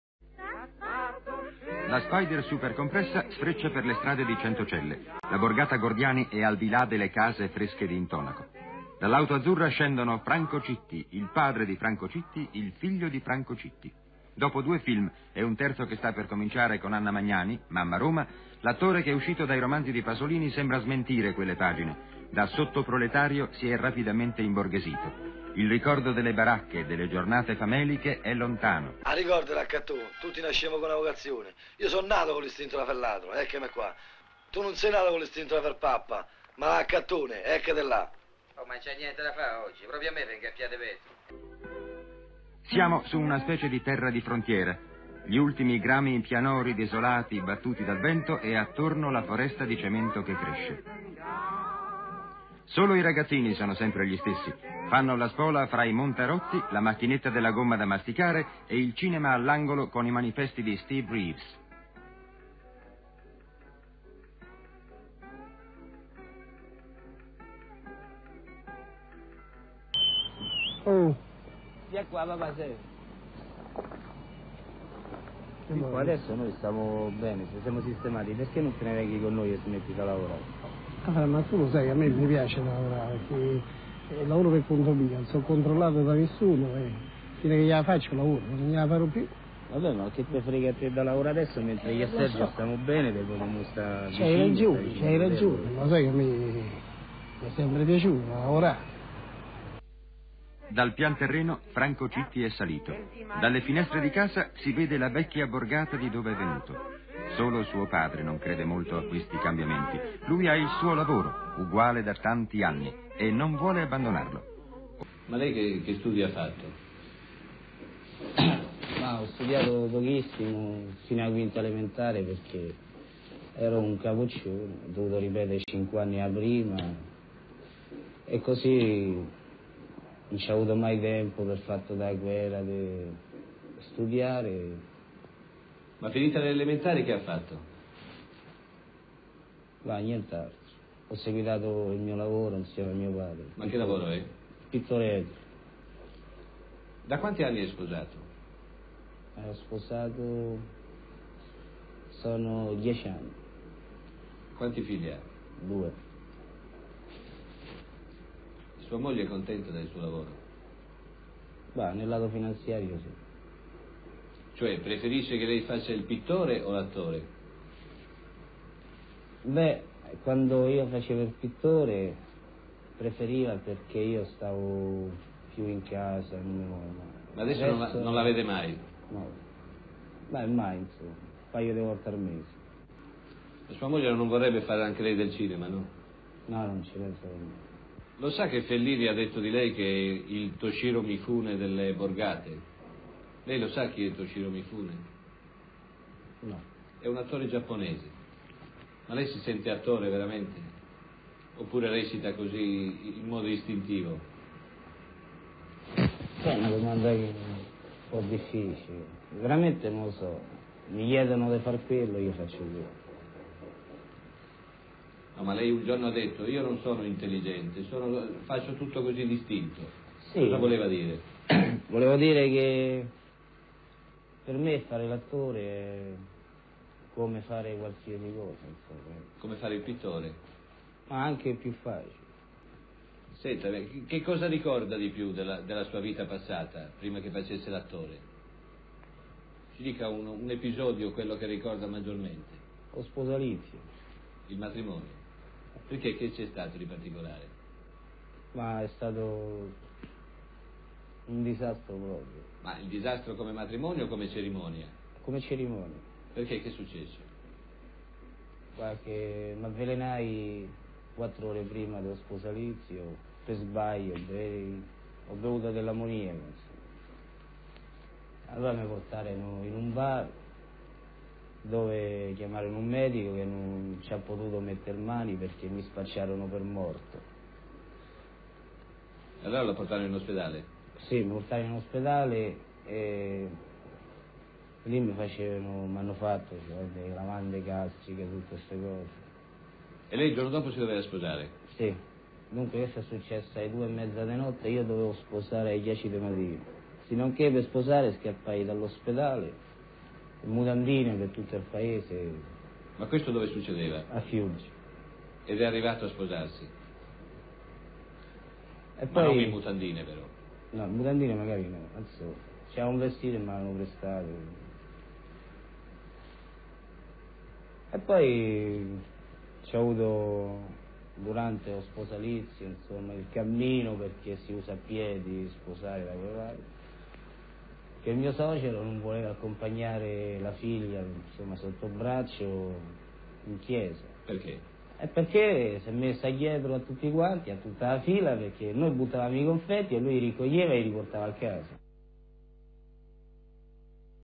Franco Citti si racconta 1963
Grazie a lui ed i ragazzi di Vita di Pasolini , quella Roma è entrata nel cinema e nei ricordi di chi ne ha visto borgate e disagi sociali...un mondo che vede in Franco Citti, la sua massima icona